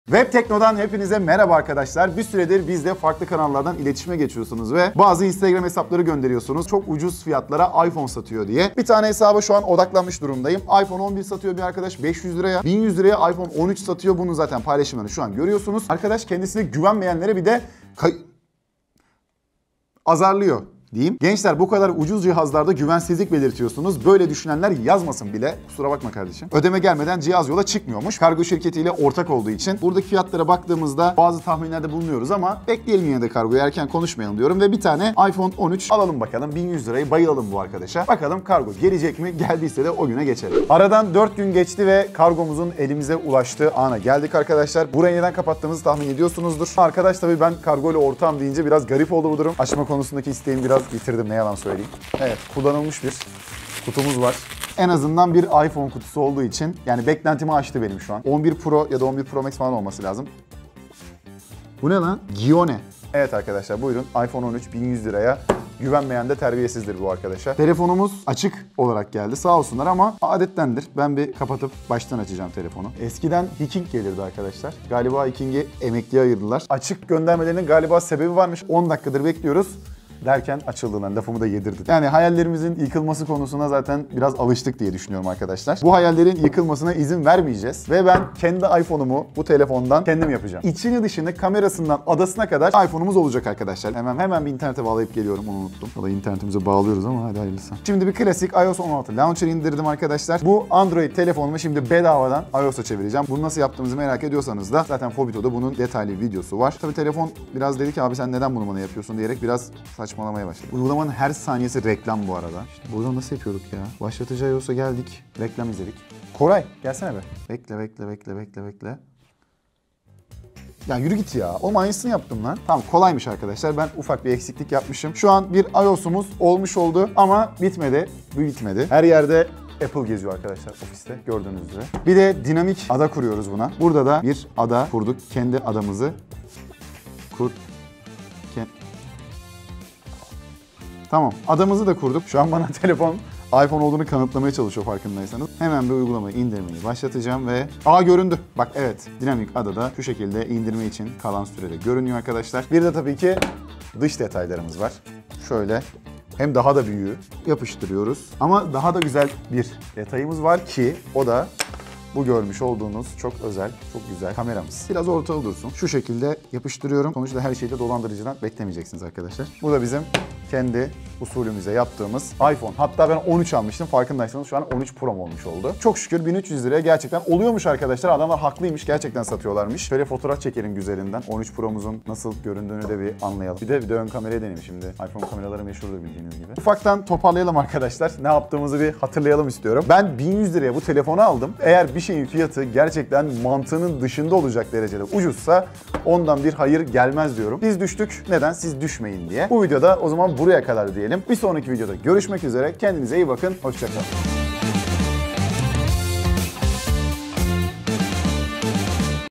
1100 TL’ye Satılan Telefonu Gerçekten(!) iPhone’a Çevirdik. (Her Şeyi Dolandırıcıdan Beklemedik)
Instagram’dan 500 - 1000 TL’ye iPhone sattığını iddia eden hesaplar, size nasıl yalan söylüyor? Bu videoda sizin gönderdiğiniz hesaplardan birini seçip 1100 TL’ye satıldığı iddia edilen iPhone 13’ten sipariş ediyor, yapılan dolandırıcılığın detaylarını açıklıyoruz. Kullanılmış iPhone 11 kutusundan, sipariş ettiğimiz iPhone 13 yerine çıkan adı sanı belli olmayan markalı telefonu da incelemeye çalıştık.

Instagram’da yanıltıcı fiyatlarla iPhone satmaya çalışan sayfaları görürseniz sayfayı Instagram’a bildirip kapanmasını sağlayabilir, eğer bu işi yaptığından şüphe ettiğiniz kişiler varsa emniyet güçlerine bildirerek daha fazla insanın dolandırılmasını engelleyebilirsiniz.

Merdan İsmail Kocaoğlu sizin için anlatıyor/deniyor!